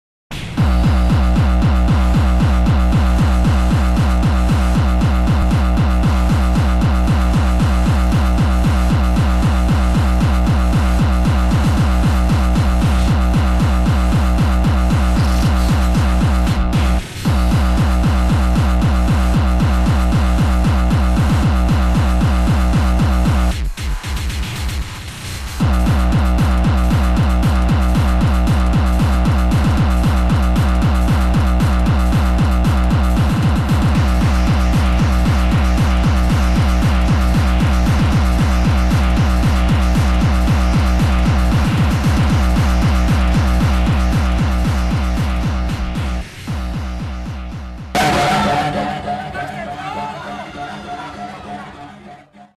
AREA INDOOR - DJ SIMON SUPPORTERS ! ! !
Mia piccolissima dedica all'AREA INDOOR DI CASTEL SAN GIOVANNI (Piacenza), realizzato con il montaggio di varie foto (e video) che ho fatto durante le mie serate lì.